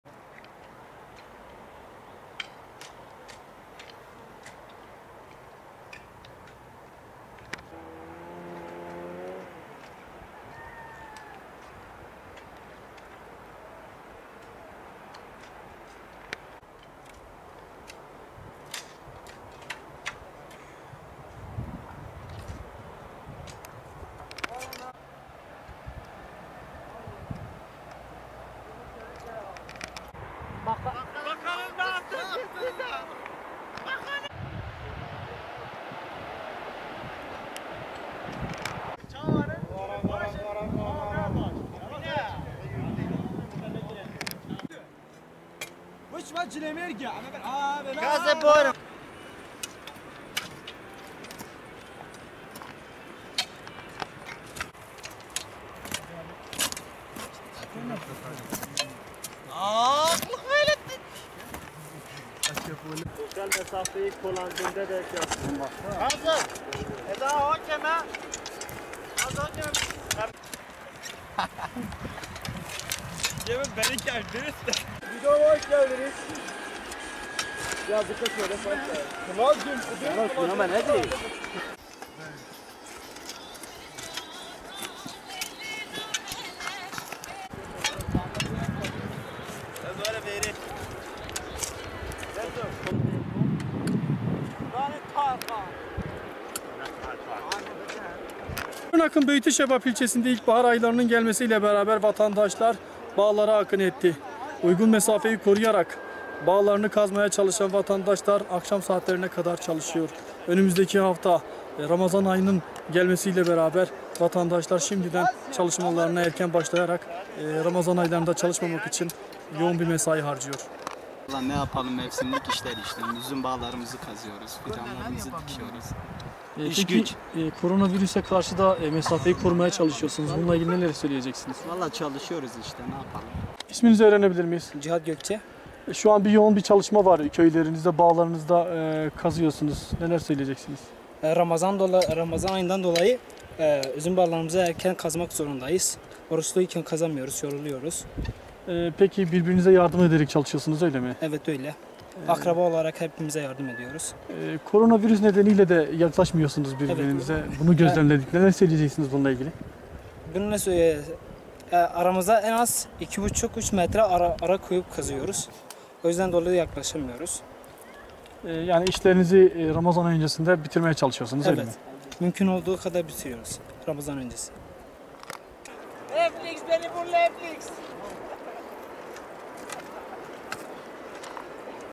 Köylüler Tarlalarını Sosyal Mesafe Kuralıyla Kazıp Ekiyorlar
Şırnak'ın Beytüşşebap ilçesinde çiftçiler tarlalarını sosyal mesafe kuralını gözeterek kazıp ekiyor.

Beytüşşebap'a bağlı Başaran köyünde ilkbahar aylarıyla birlikte üzüm bağlarını budayıp kazmak zorunda kalan köylüler korona virüs salgını nedeniyle birbirine yardım ederek işlerini Ramazan öncesi bitirmeye çalışıyor. Sosyal mesafeye uyarak 2 metre arayla her gün bir üzüm bağını kazmaya çalışan köylüler, salgın nedeniyle birbirine yaklaşamıyor. Uygun mesafeyi koruyan gençler sabahın ilk ışıklarıyla başlayıp akşam saatlerine kadar toprağı kazıyor. Birbirlerine yardım ederek işlerini Ramazan öncesi bitirmeye çalışan köylüler oruç tutacakları için bağ bahçe işlerini erkenden bitirmeye çalışıyor.
Halit Gökçe isimli bir vatandaş ise" virüs nedeniyle mesafelerini koruyarak çalıştıklarını" söyledi. Gökçe, "Ramazan öncesi işlerimizi bitirmeye çalışıyoruz, mecburuz. Üzüm bağlarını kazamazsak hasadı yapamayız. Mesafeyi koyarak çalışıyoruz" dedi.
Cihat Gükçe adında bir genç ise her gün 1 üzüm bağını kazdıklarını ifade etti. Cihat Gökçe, "Günlerdir kazma ve küreklerle üzüm bağlarını kazıyoruz. Virüs nedeniyle birbirimize yaklaşamıyoruz. 2 metre arayla bağları kazıyoruz. Şimdiye kadar kardeşlerim ve amca oğlumla beraber 6 üzüm bağını kazdık" diye konuştu.
Vahit Gökçe de "Her yıl bu vakitlerde çalışıyoruz. Bahar aylarında tüm işlerimizi bitirip Ramazan ayı öncesi birbirimize yardımlaşarak işlerimizi bitiriyoruz" şeklinde konuştu.